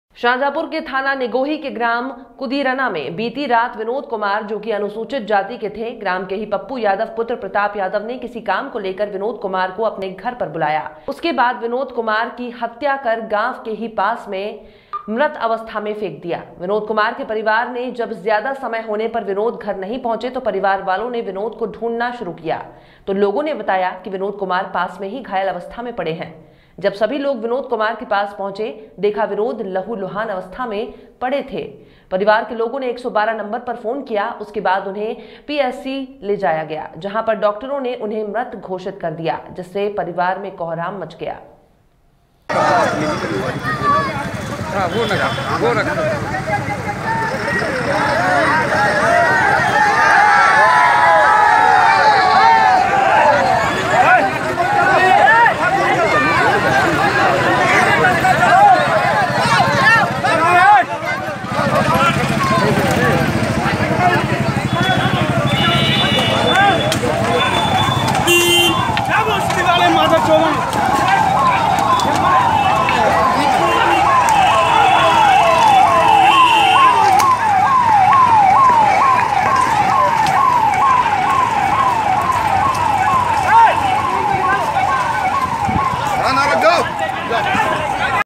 पुरानी रंजिश के चलते अनुसूचित जाति के विनोद कुमार की हत्या ..UFT NEWS 24
शाहजहाँपुर के थाना निगोही के ग्राम कुदिरना में बीती रात विनोद कुमार जो कि अनुसूचित जाति के थे ग्राम के ही पप्पू यादव पुत्र प्रताप यादव ने किसी काम को लेकर विनोद कुमार को अपने घर पर बुलाया उसके बाद विनोद कुमार की हत्या कर गांव के ही पास में मृत अवस्था में फेंक दिया विनोद कुमार के परिवार ने जब ज्यादा समय होने पर विनोद घर नहीं पहुंचे तो परिवार वालों ने विनोद को ढूंढने शुरू  किया तो लोगों ने बताया की विनोद कुमार पास में ही घायल अवस्था में पड़े हैं जब सभी लोग विनोद कुमार के पास पहुंचे देखा विनोद लहूलुहान अवस्था में पढ़े थे परिवार के लोगों ने 112 नंबर को फोन किया उसके बाद उन्हें पीएससी ले जाया गया जहां पर डॉक्टरों ने उन्हें मृत घोषित कर दिया जिससे परिवार में कोहराम मच गया
हमसे जुडने के लिए इन लिंक्स पर फॉलो करें: